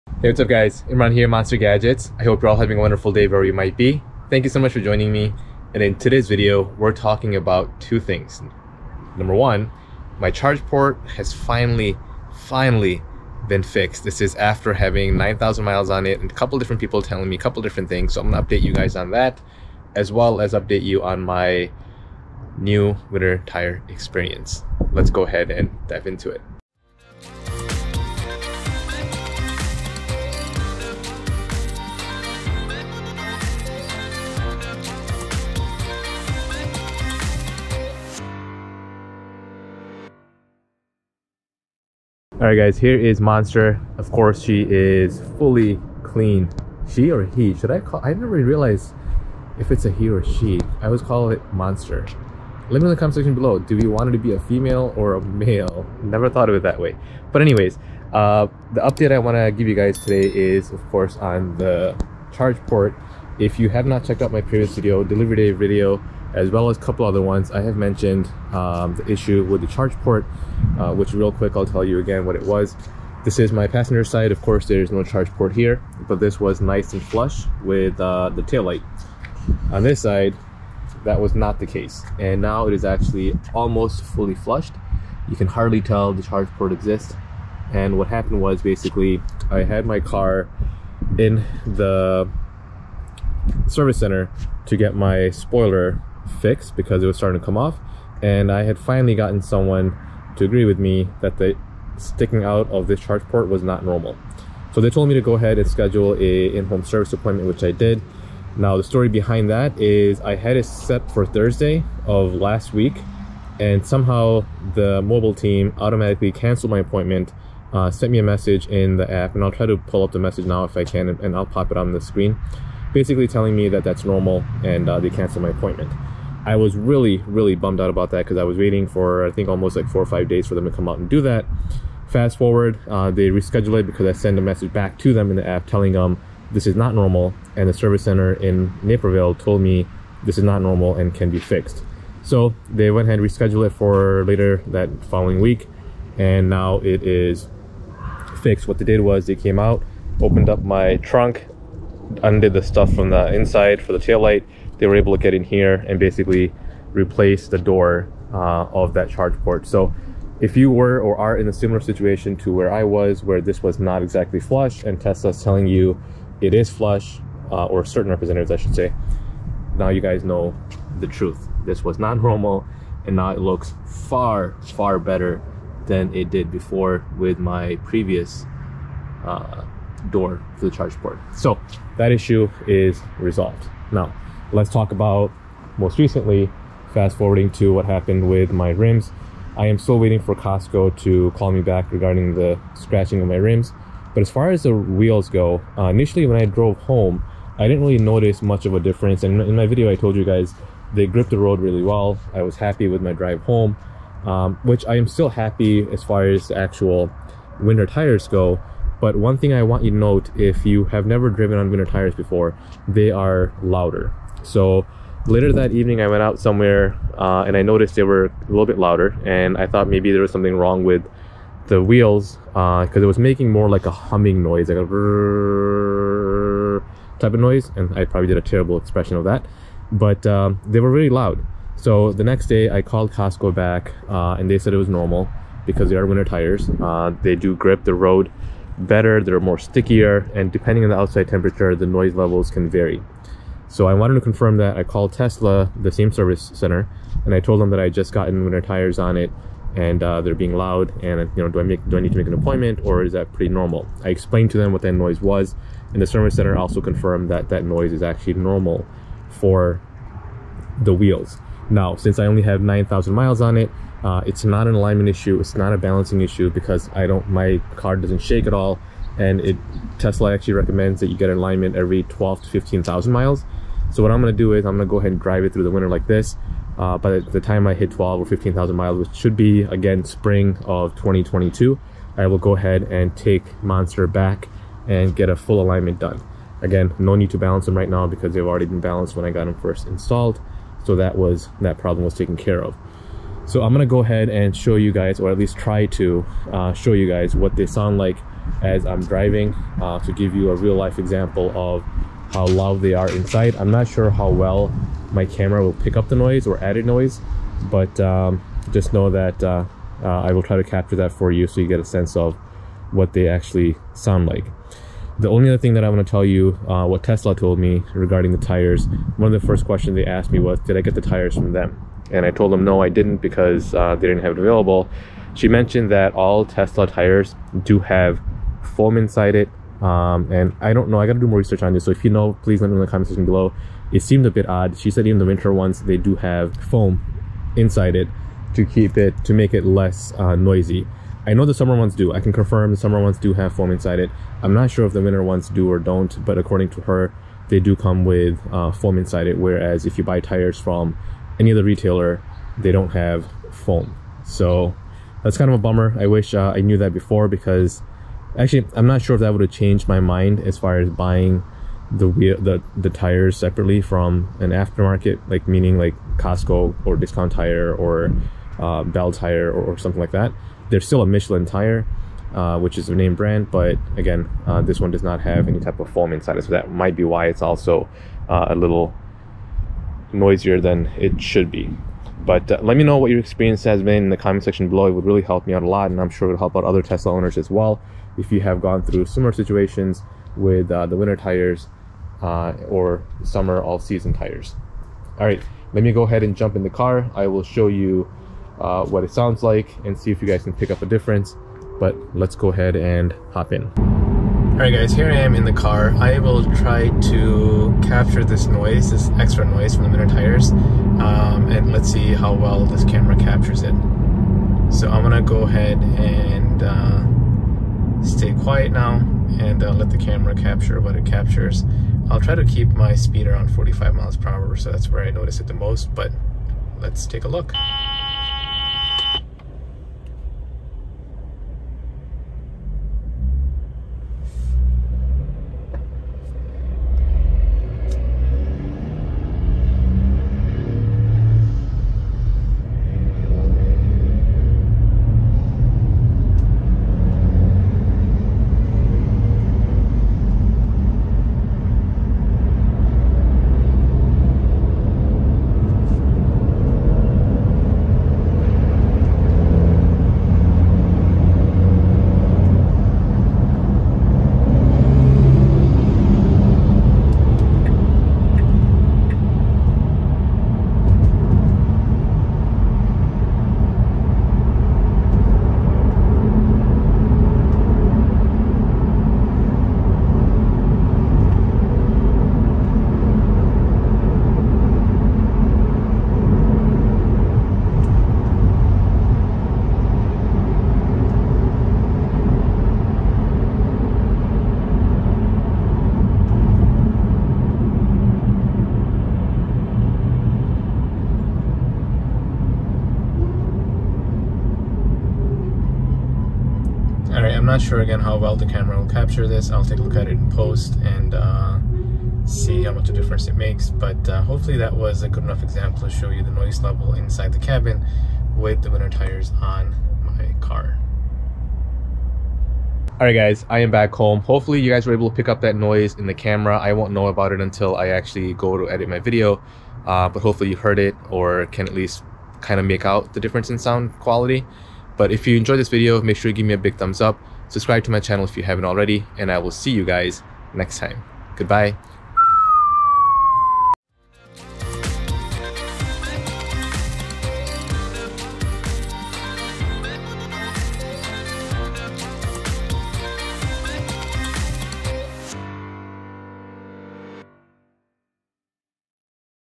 Tesla Model 3 - Charge Port Fixed and Winter Tire Update
In this video, I will talk about finally getting my charge port gap fixed as well as my experience with getting Winter /Snow Tires for my 2021 Tesla Model 3 Performance. I bought the Michelin Alpin PA4 Winter Tires in my size of 235/35/20.

Link to the tires

If you are in the market to order a New Tesla, please use my link to get 1,000 miles of free supercharging from Tesla!

0:00 Video Intro
0:26 MonsterGadgets Intro
0:42 Charge Port
3:03 Winter Tires
6:43 Noise Test
8:42 Wrap up